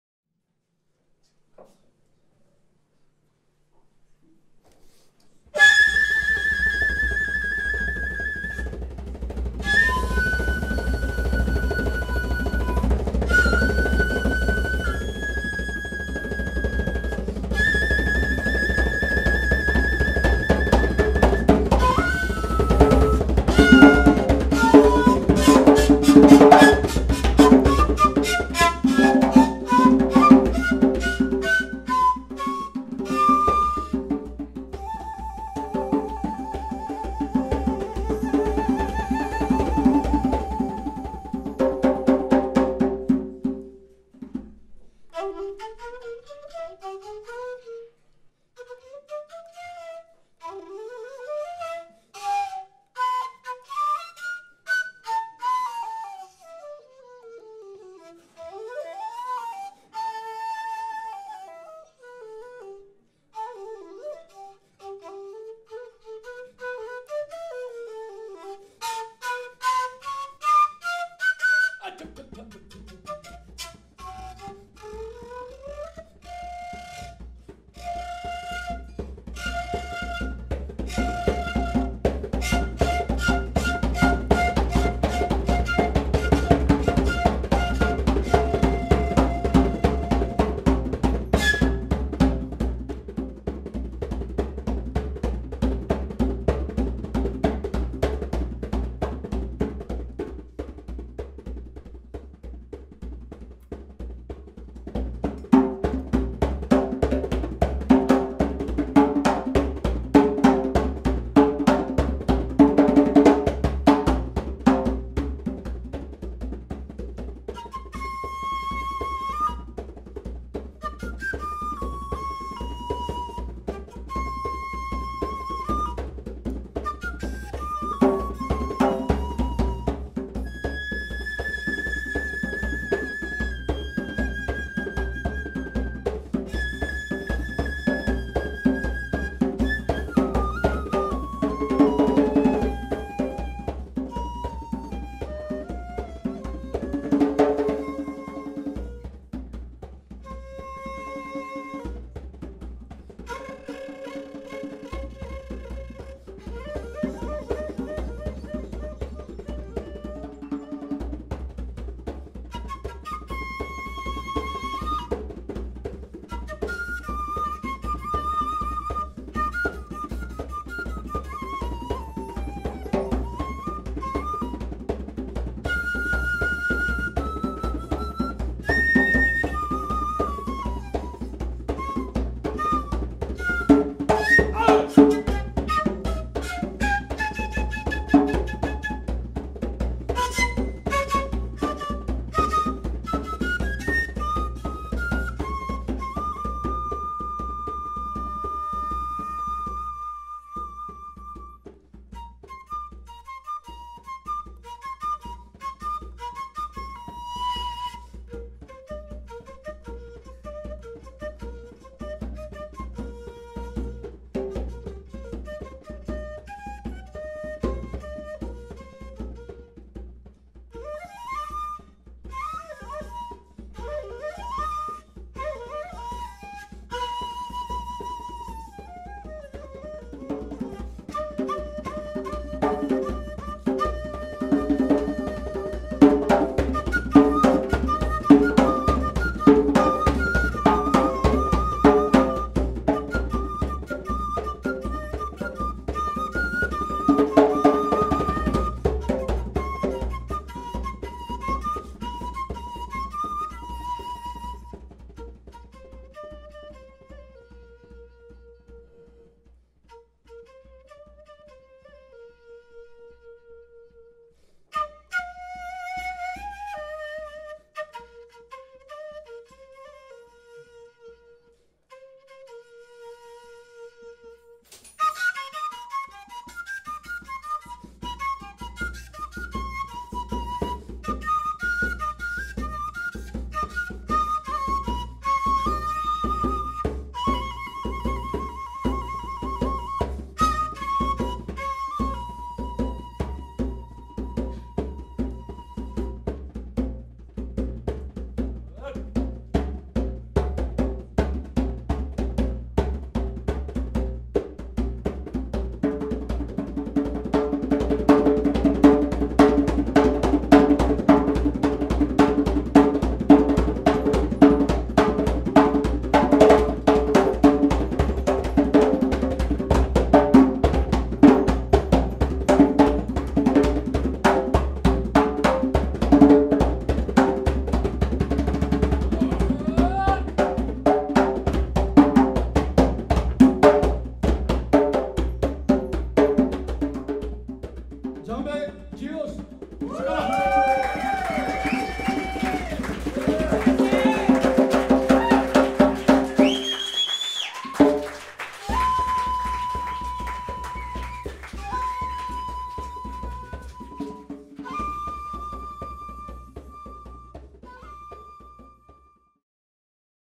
FUJIN RAIJIN @ MONARCH
FUJIN RAIJIN IS AN IMPROVISATION DUO BAND. SIMPLE.HOT.ANCESTRAL.HARD & EXOTIC!!

DAIYOSHI KIZAN      - SHAKUHACHI + BELL
KIYOSHI FUJIKAWA - DJEMBE